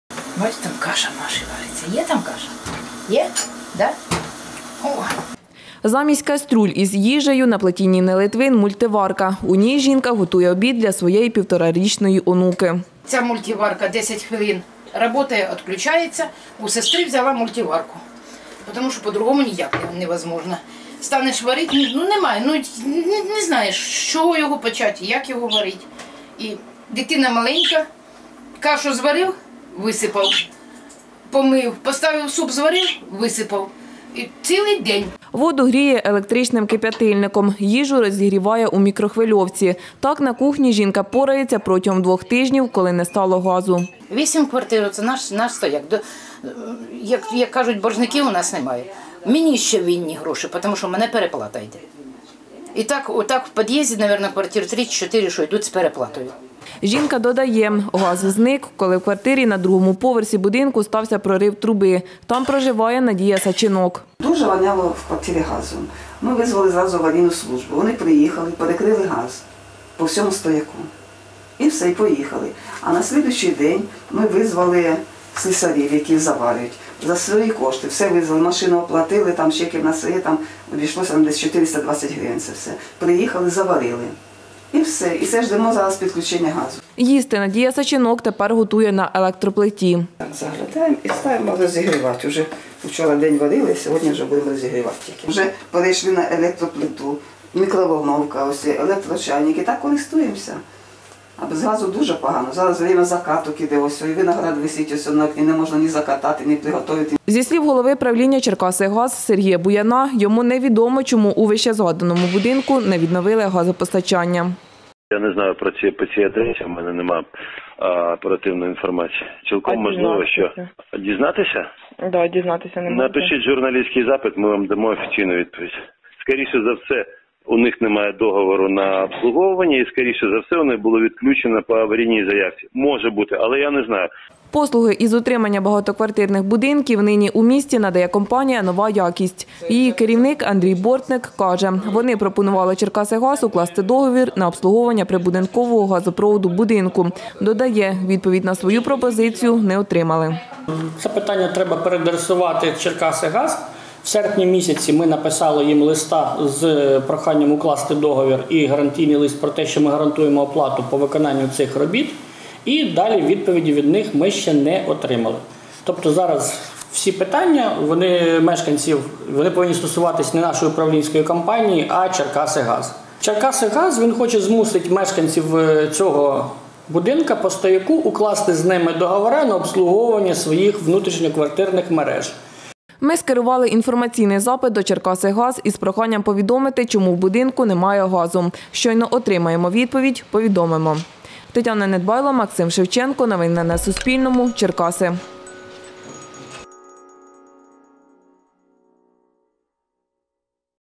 Два тижні без газу мешканці восьми квартир у Черкасах
Два тижні без газу мешканці восьми квартир у будинку за адресою Тараскова, 5 у Черкасах. Про це журналістам Суспільного повідомила місцева мешканка Надія Саченок. Із її слів, 21 серпня майстри підприємства «Черкасигаз» відімкнули газ у її помешканні через несправність крану. Досі газопостачання не відновили.